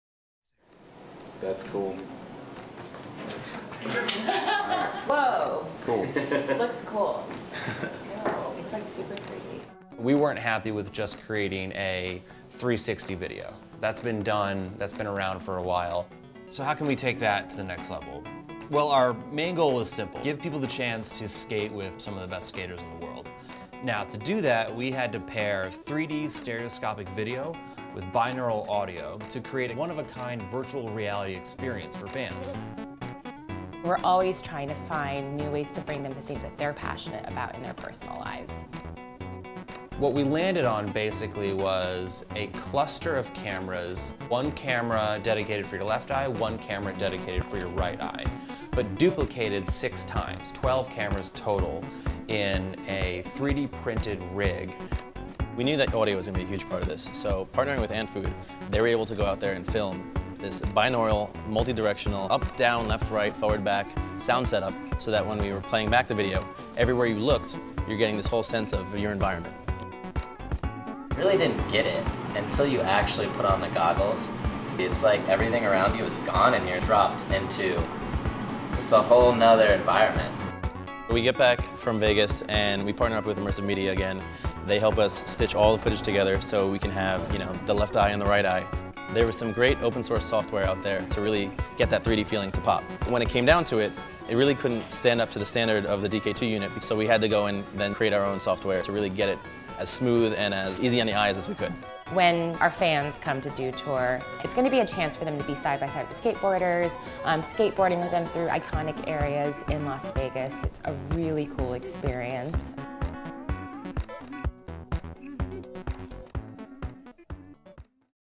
4A's Partner Awards, presented by MediaLink - Small Agency, Technology Partnerships
Technology Partnerships, Small Agency Winner
Firstborn
"Dew VR Skate Experience"
Partners: Mountain Dew, Brain Farm, Antfood, Immersive Media